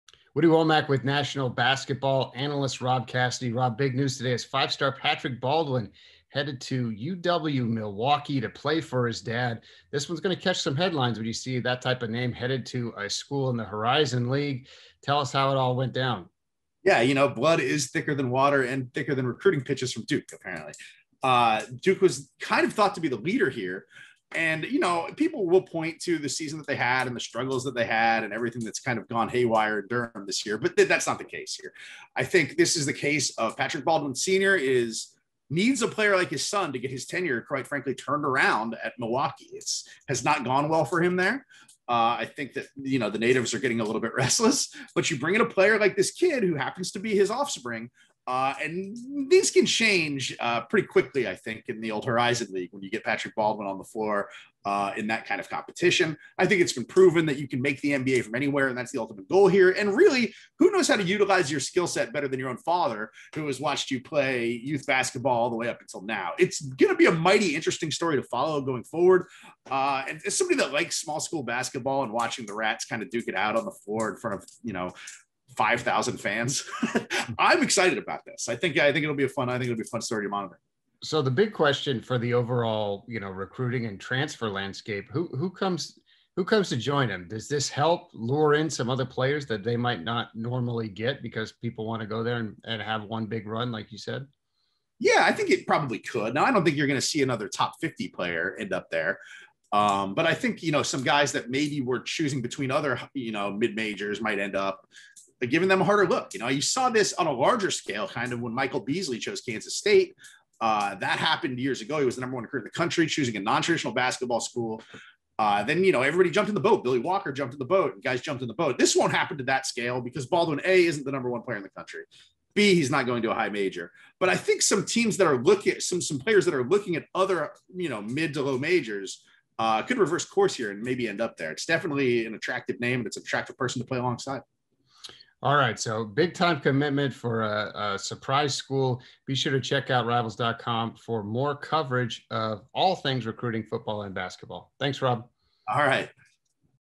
Analysis Five-star Patrick Baldwin land at UW-Milwaukee
Rivals.com's Rob Cassidy and Woody Wommack breakdown how UW-Milwaukee landed five-star Patrick Baldwin and what it means for the program.